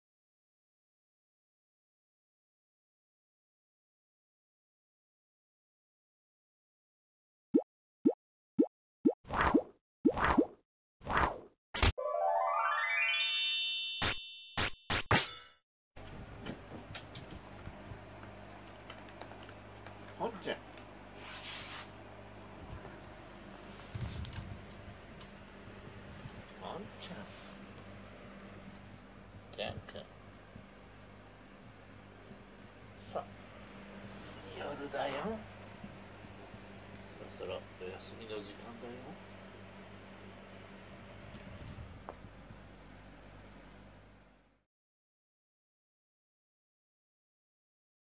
ポンちゃんが元気になって走るのをジャンくんは見てる【Jean & Pont 3569】2024/07/02 保護猫ジャンけんポン
猫ってなんか急に元気になって走り回ることってあるよね。
トイレハイっていうのもあるけど、なんか急に。
絵今？ってことあるね。
ジャンくんはそれを見つめています。